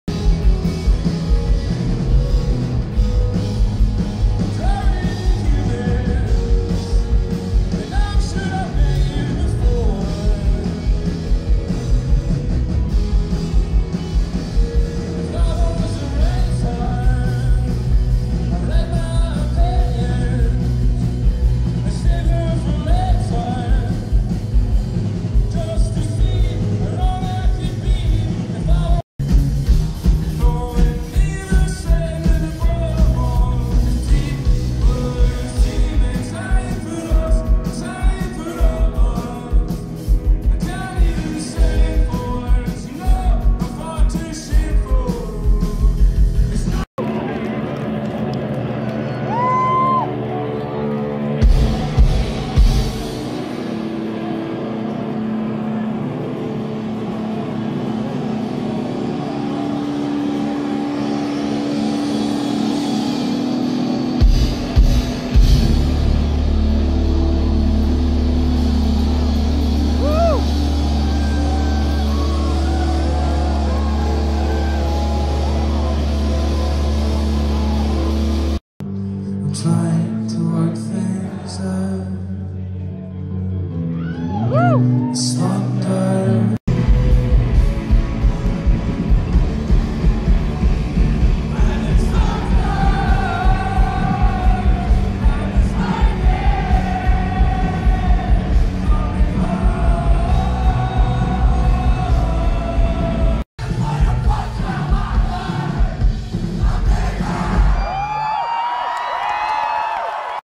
We Were Promised Jetpacks at the El Rey Theatre Feb 21 2014
Here are various clips I recorded during the We Were Promised Jetpacks concert at the El Rey Theatre in Los Angeles February 21, 2014. This was the first show on their tour. This was filmed with a Nokia Lumia 920.